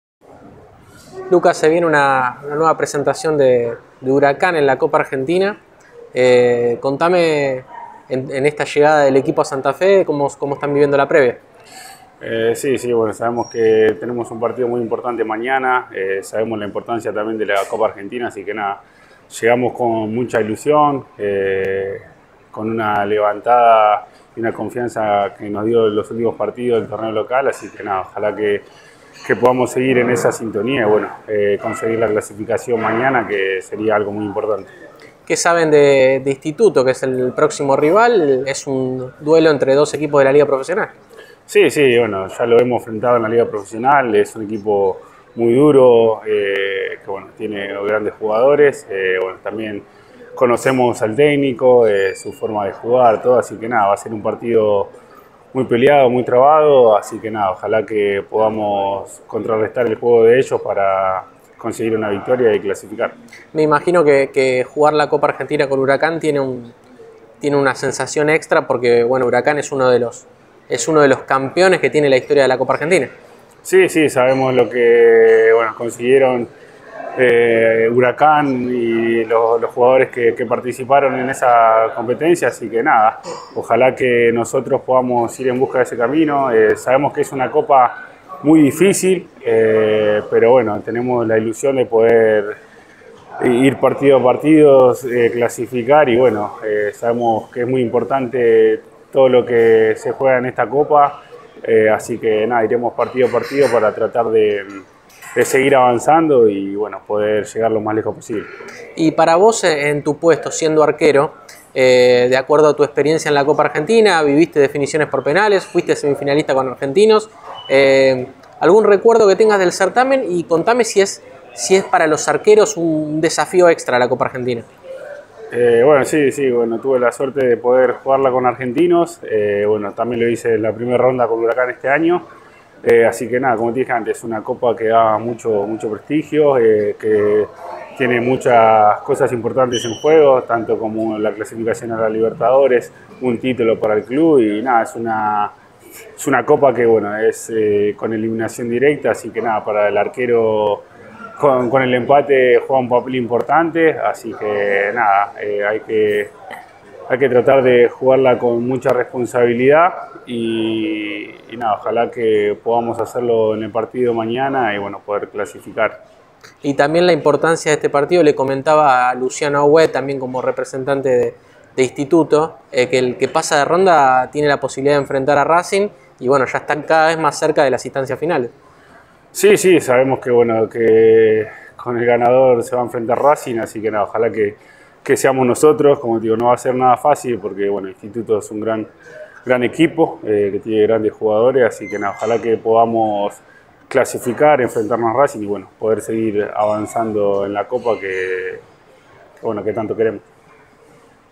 Lucas Chaves - Huracán
El arquero habló con el Sitio Oficial de la Copa Argentina AXION energy, en la previa al cruce contra Instituto en Santa Fe, por los 16avos de Final.